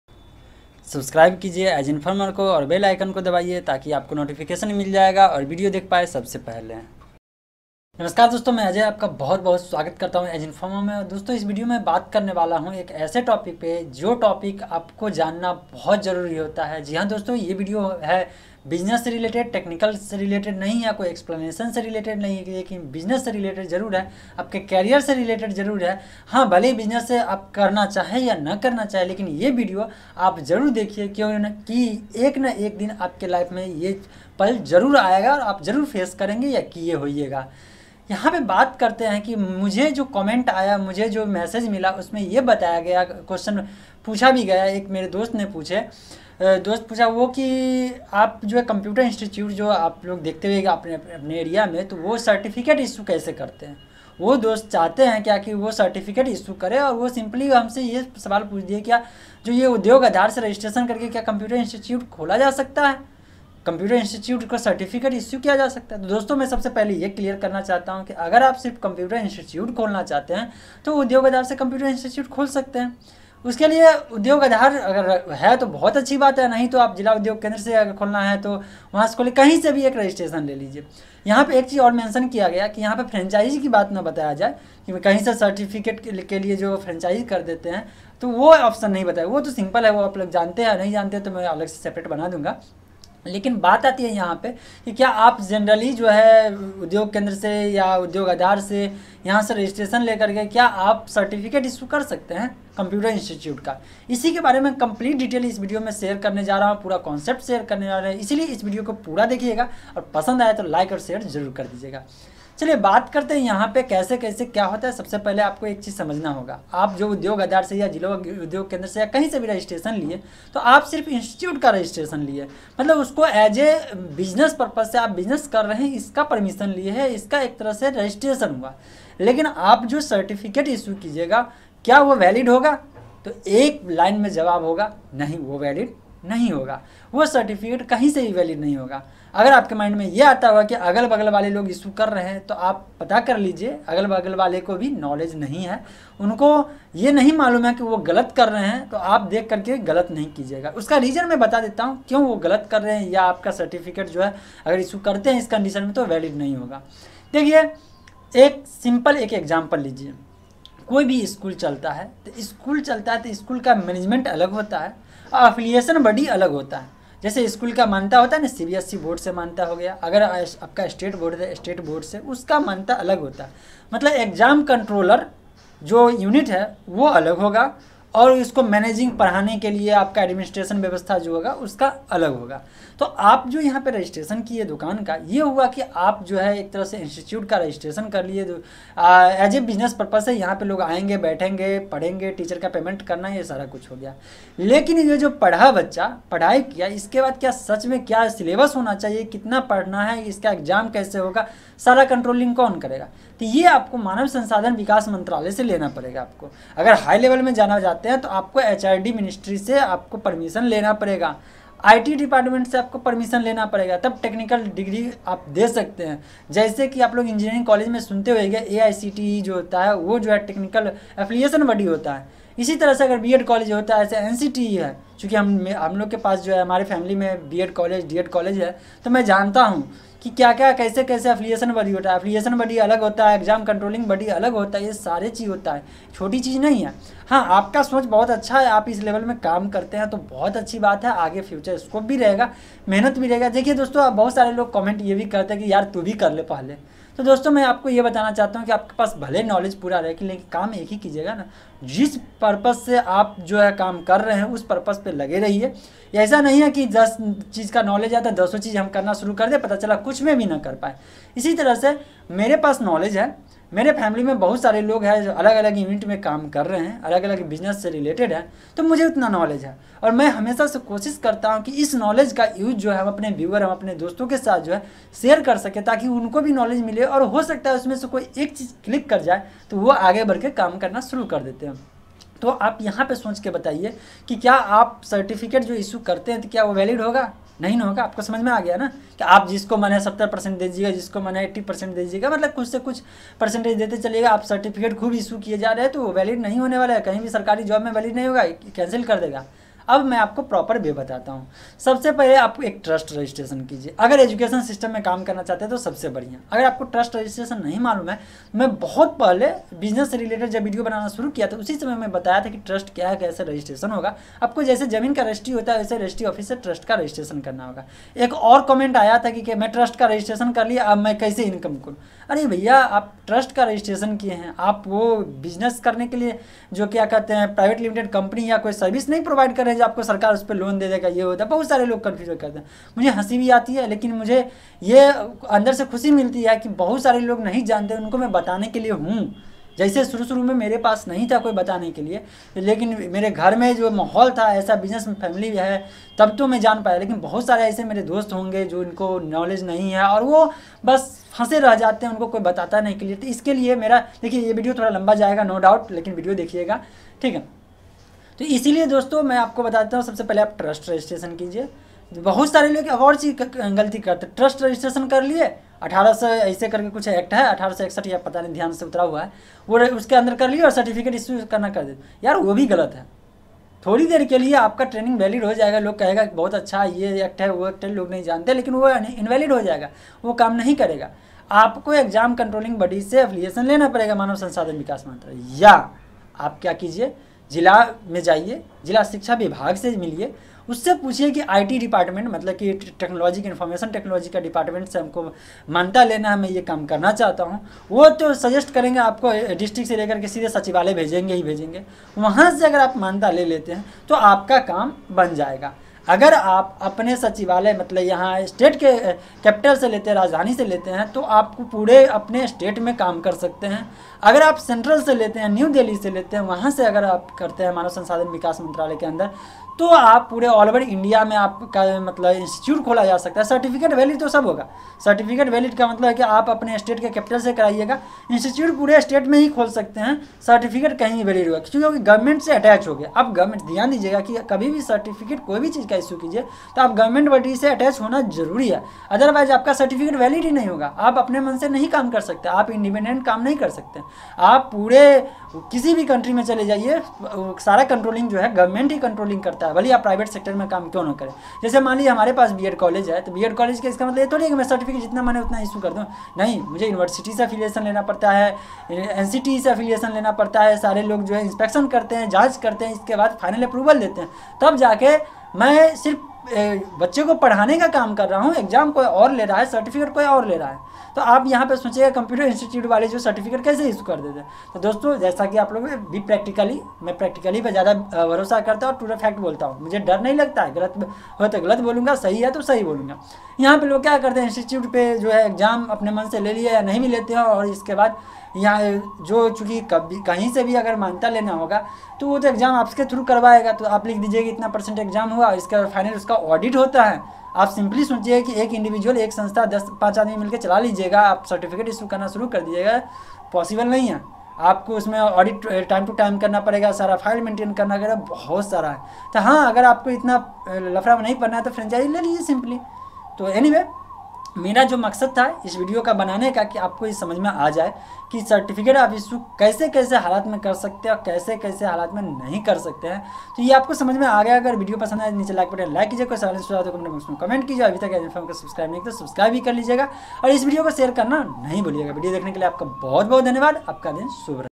Business - How to provide legal Certificate of Computer Courses ?
Hello dosto, agar aap computer institute run kar rahe hai aur aap agar apna kghud ka certificate generate karna chahte hai to ye video dekhiye taki complete details samajh me aa jayega.
What about your opinions? tell me in comment.

About : AS Informer channel  contains daily tech news, How to guide and review  with lot of technology concept.